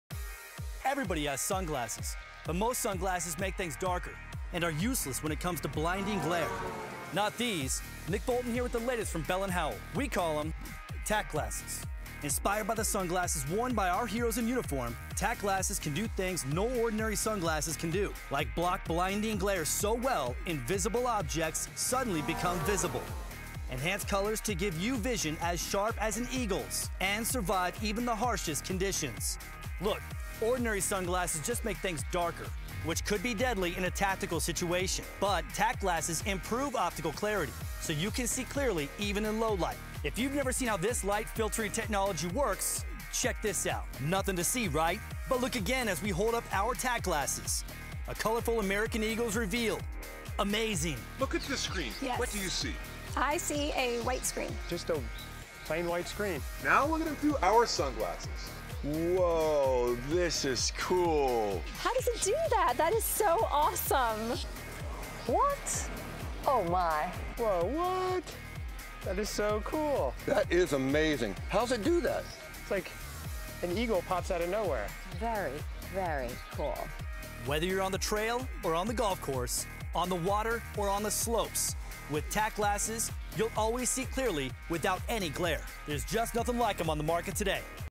Tac Glasses Commercial As Seen On TV
Block glare, enhance color and improve optical clarity with Tac Glasses as seen on TV.